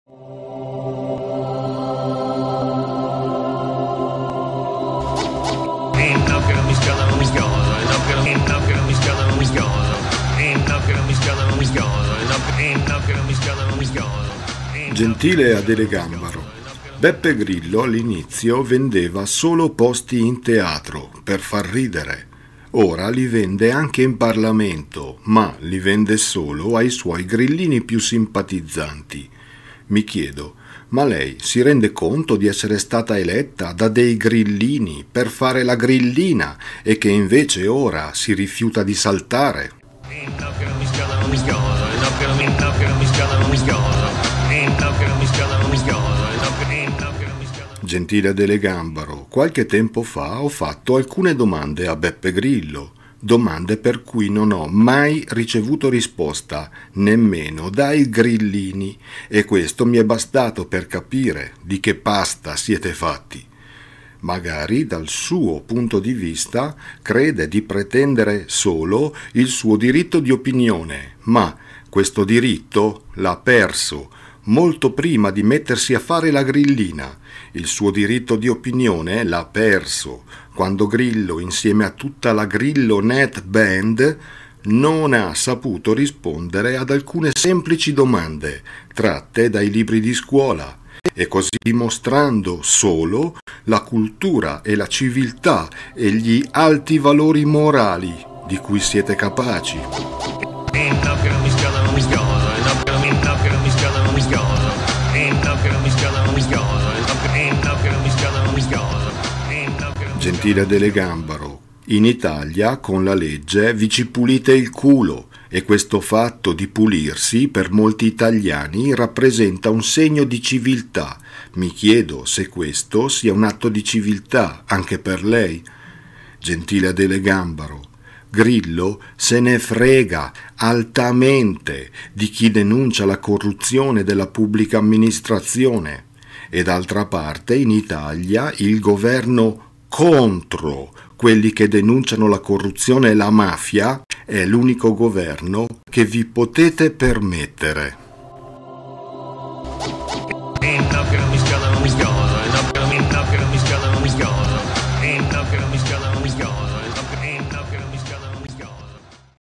Grillo: E no che non mi schiodo
Gentile Adele Gambaro, magari dal suo punto di vista crede di pretendere solo il suo diritto di opinione, ma questo diritto lei l'ha perso molto prima di mettersi a fare la grillina. Il suo diritto di opnione lei l'hai perso quando Grillo insieme a tutta la Grillo-Net-Band non ha saputo rispondere ad alcune semplici domande, tratte dai libri di scuola...  E cosi' dimostrando la cultura e la civilta' e gli alti valori morali di cui siete capaci...

Grillo se ne frega altamente di chi denuncia la corruzione della pubblica amministrazione... E d'altra parte in italia il governo contro quelli che denunciano la corruzione e la mafia della pubblica aministrazione e' l'unico governo che vi potete permettere

ALTRI RIFERIMENTI